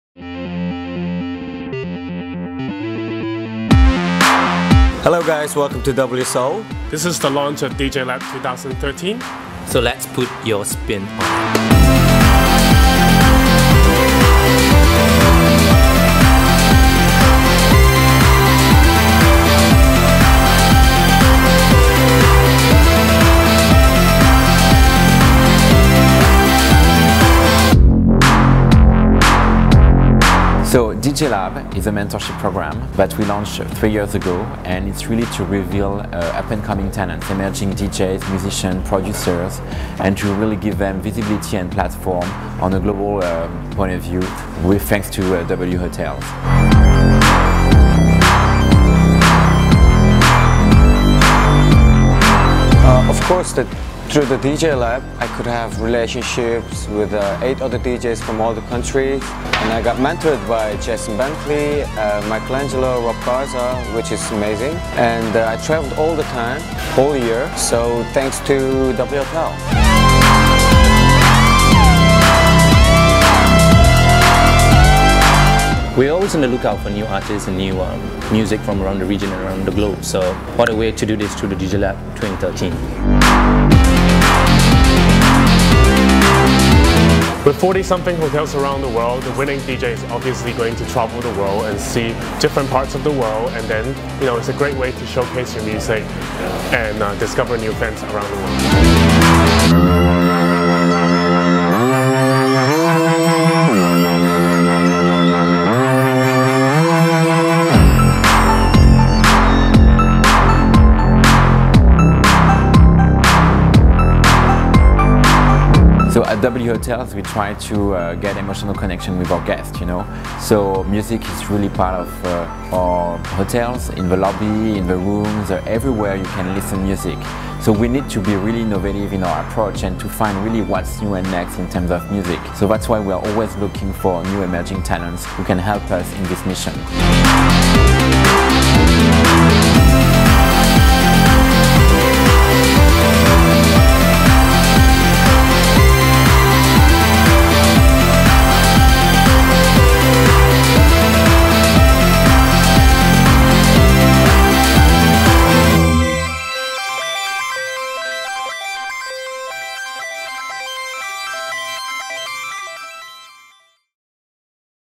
Gio Vanhoutte - DJ Interview - W Seoul
Korean-Belgian DJ Gio Vanhoutte is one of the uniqueness character in the international club scene. Gio has enlivened thousands of people in every corner of the globes by his both Asian and European sensibilities as well as powerful performances.

Gio Vanhoutte started electronic music and DJing in Belgium since 2006. While he played as resident DJ in Lime Club, DecaDance and Culture Club in Gent (Belgium), which has turned out the great artists like The Subs, 2 Many DJs, etc, he could quickly develop his skills and upfront approach to making and playing music to turning him into a global phenomenon. He also has influences of his very close friends like Maxim Lany, Nathan Fix, NEON, Ante Perry, Ramon Tapia, Noir, and Martin Eyerer.

Gio later hold chance to playing in South Korea where he spend his childhood. After several times playing, he got instantly fascinated by Asian club scene. He believes that his passion to conquer Asian club scene made his mixing skills more energetic.

At present, Gio represents Asia/Pacific for W Hotels Worldwide after he won the competition called W Hotels & Burn Studios DJ Lab 2012. His advantage is playing 2 continental different genres of house music. When in Europe, he goes deep & sexy. When in Asia, he burns the dance floor down with Progressive & Electro.

For the next step, Gio is heading to his dream as producer by the international exchange with worlds big artists and Belgian connection. He also spread his musical worldview through the Taiwan magazine PPAPER as guest editor.

As a international DJ, Gio is traveling all around the world and playing remarkable sounds in South Korea, Singapore, Taiwan, Thailand, Indonesia, Hong Kong, Russia, France, Belgium, Spain, America, Mexico and more!